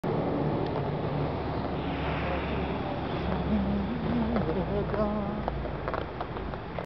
Gasoline rainbow.
Finally, an aesthetic use for fossil fuel. All the colours of the spectrum.